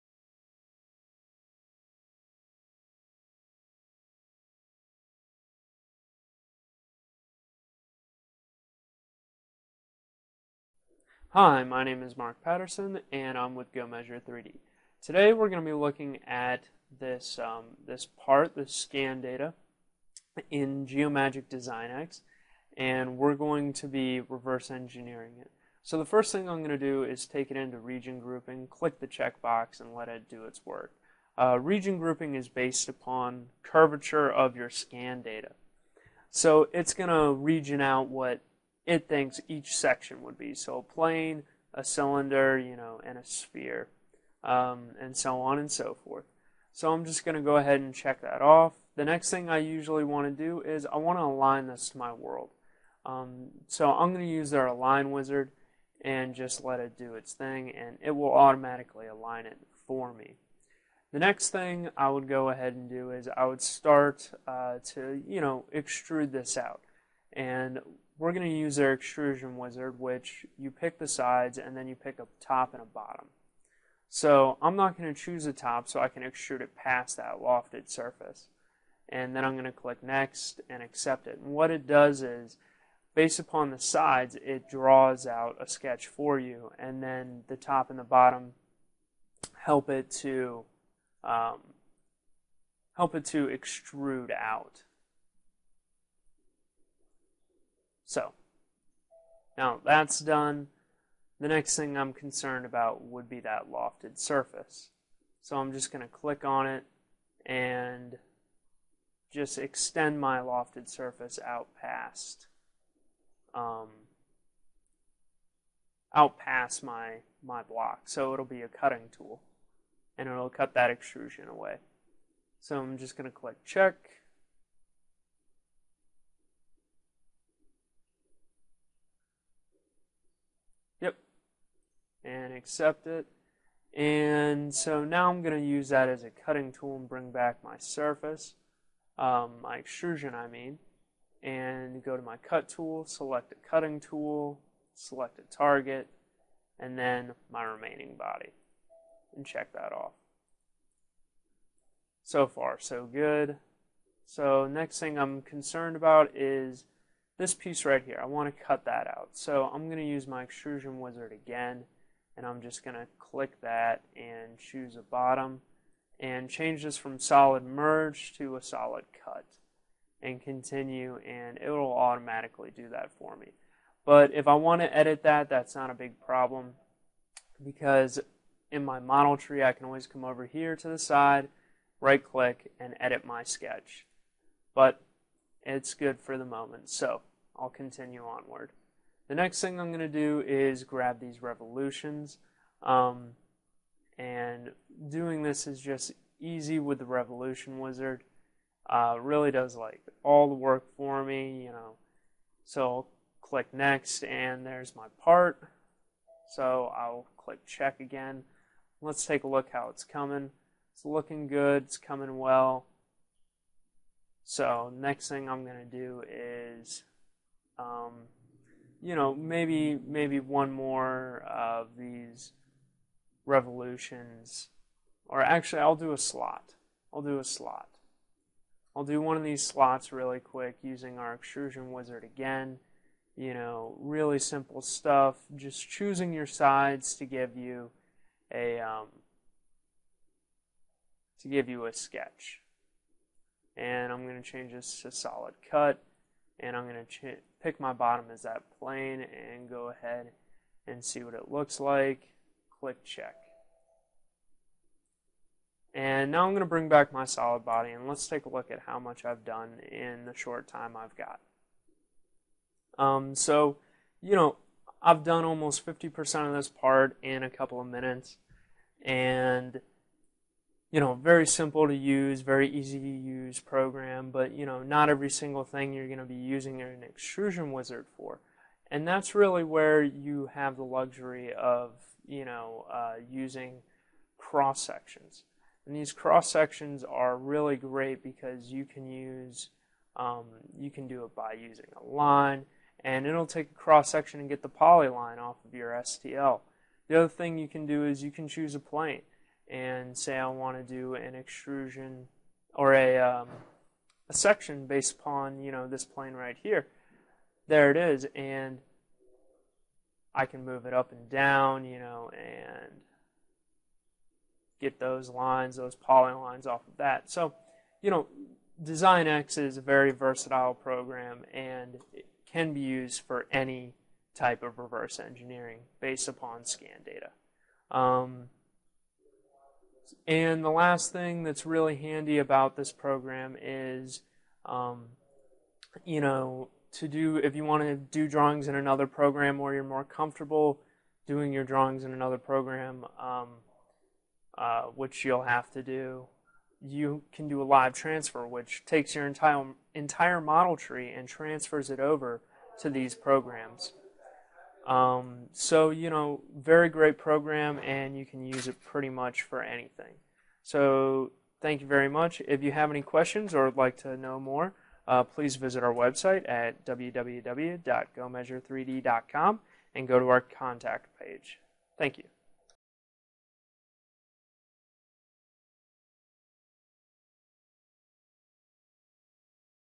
Geomagic Design X Getting Started Tutorial
Mark from GoMeasure3D gives a brief look at how Geomagic Design X works and demonstrates some of the software's features. Geomagic Design X software combines 3D scan data with history-based CAD so you can create feature-based, editable solid models compatible with your existing CAD software.